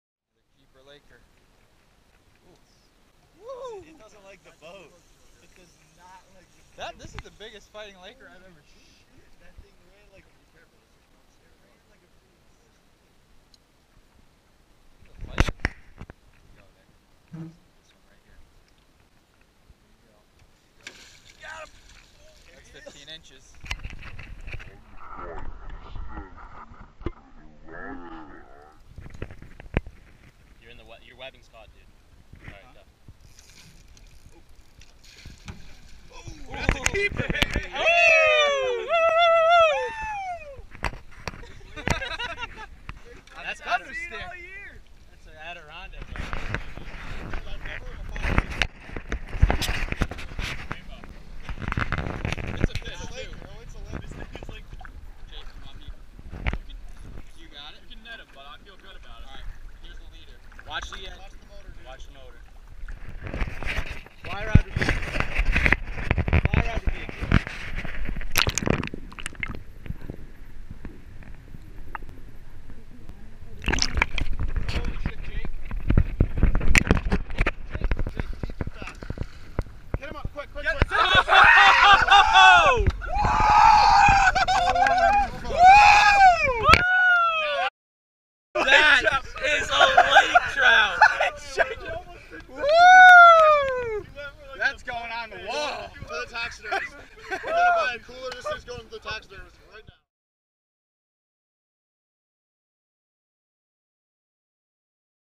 Lake Placid Lake Trout Fishing
Memorial Day weekend trolling on beautiful Lake Placid NY. The lake trout bite was on! We caught them on crank baits and trolling flies. The big one at the end measured 32 inches!